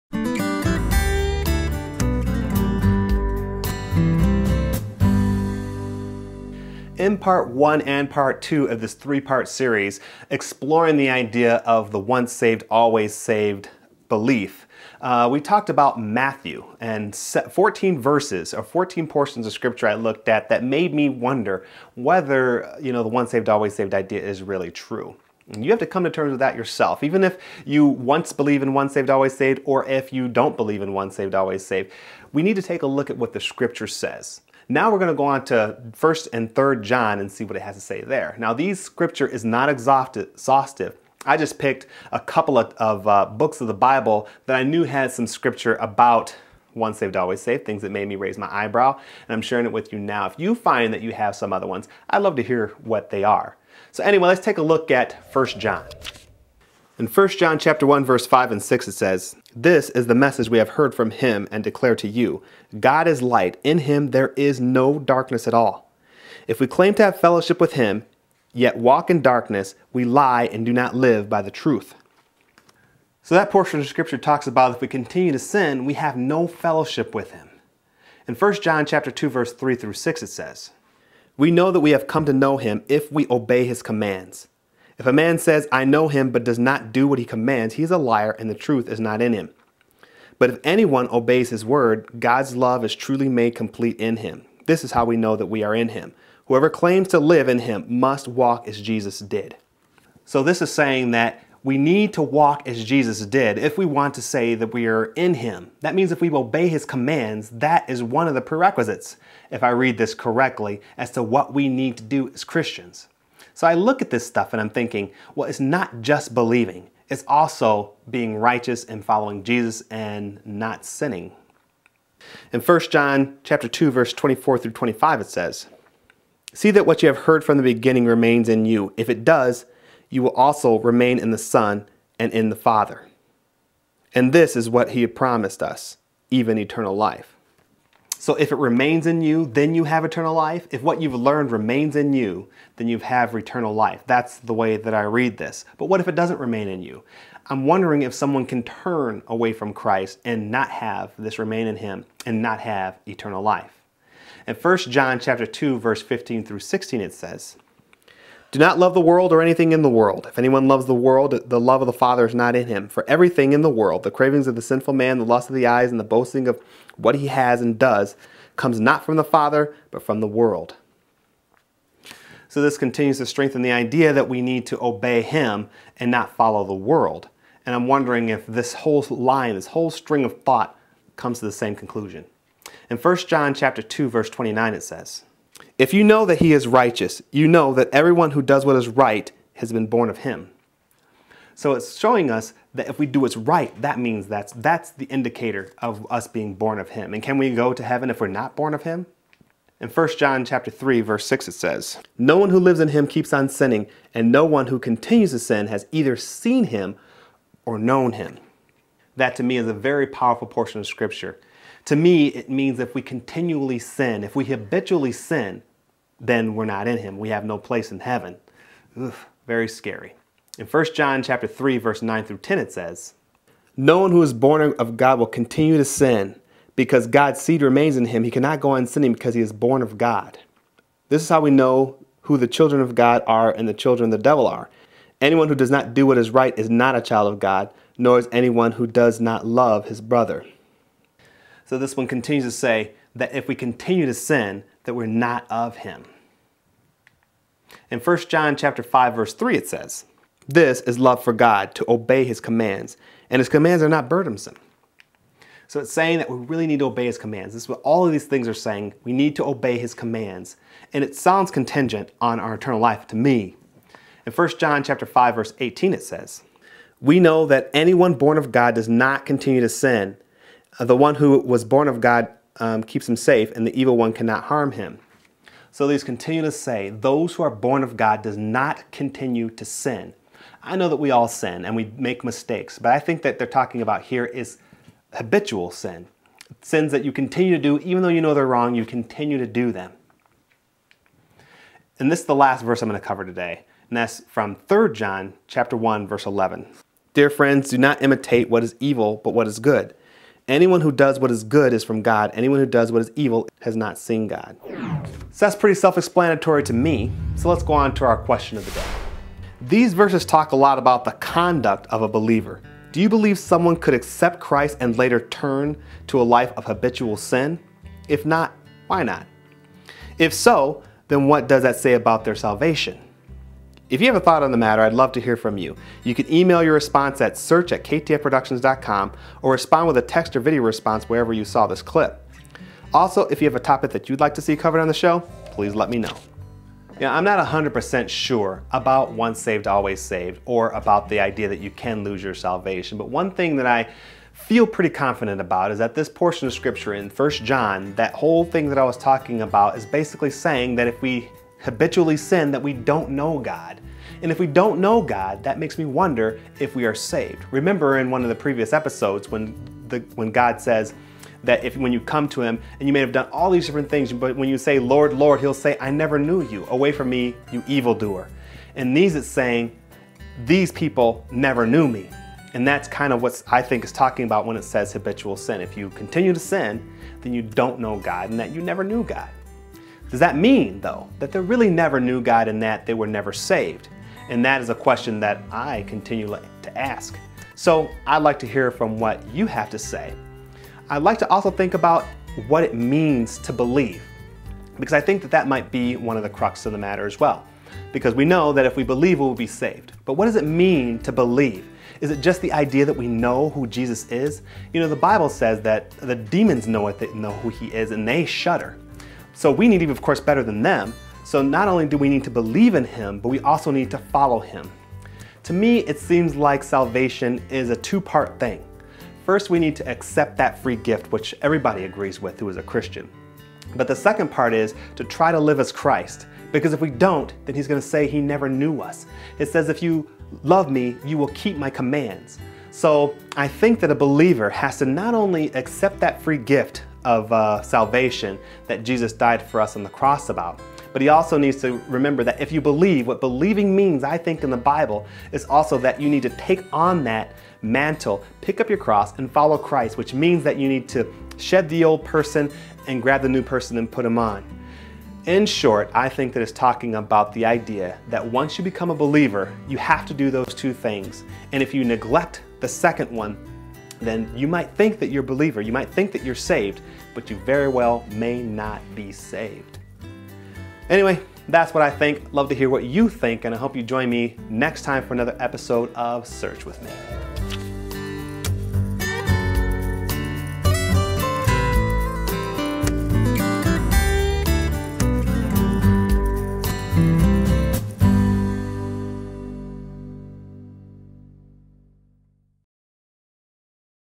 49 OnceSaved? Part 3 of 3
I explore the belief that once a believer is saved that they can never lose their salvation. Join me, and post your beliefs on this controversial topic.